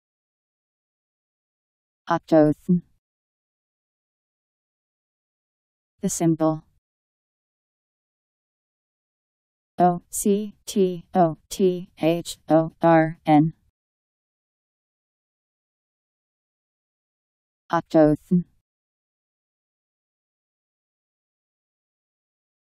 Octothorn Meaning
Video shows what octothorn means. , the symbol #..  Octothorn Meaning. How to pronounce, definition audio dictionary. How to say octothorn. Powered by MaryTTS, Wiktionary